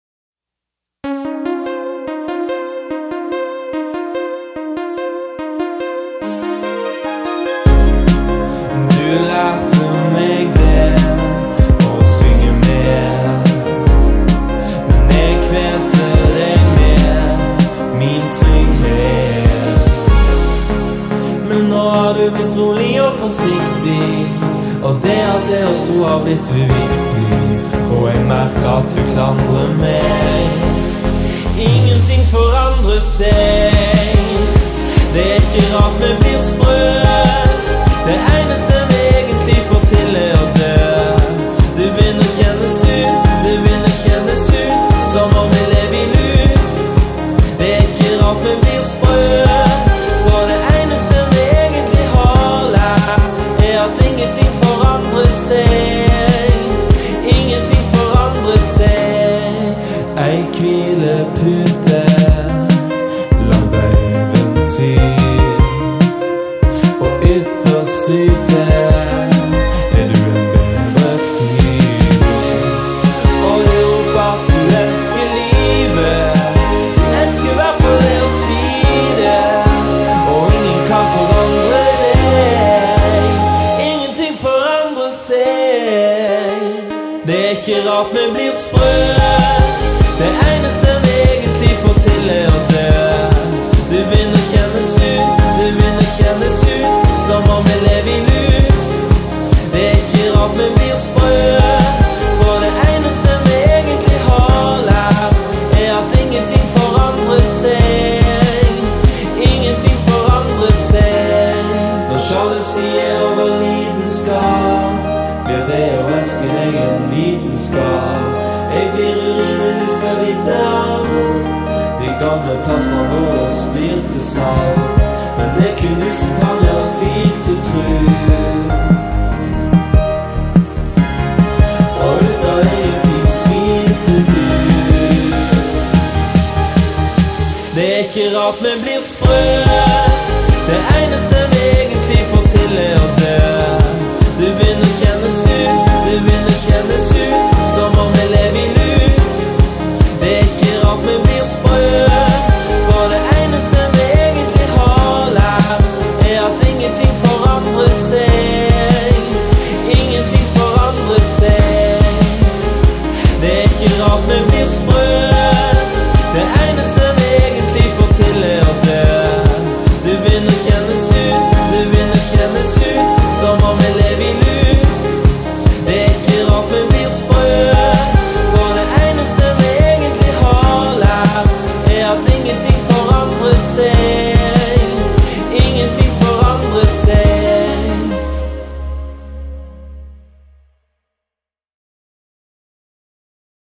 First Month with DJI Phantom 2 Vision+
Mostly shot in Kristiansand, Norway.